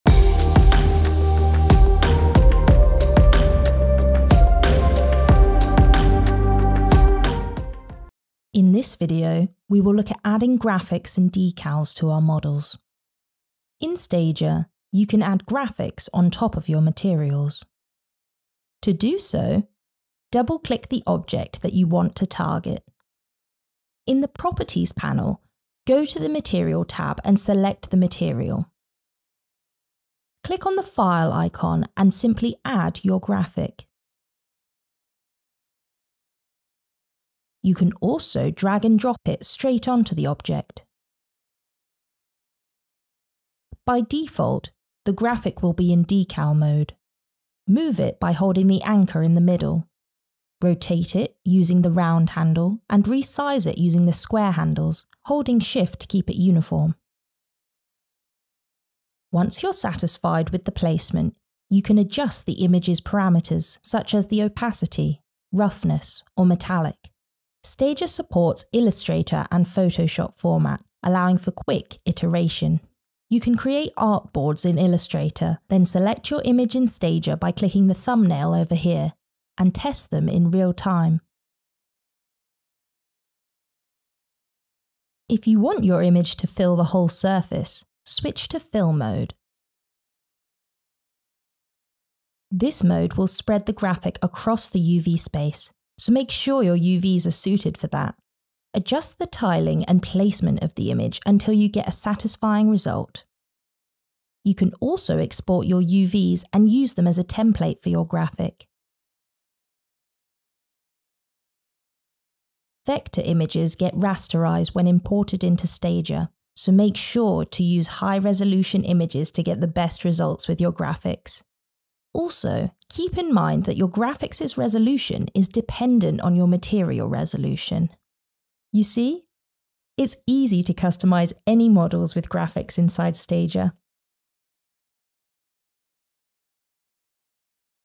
Adding Graphics with Substance 3D Stager | Adobe Substance 3D
In this video, we will go over how you can apply custom graphics to your objects and use artboards to test your ideas on the fly.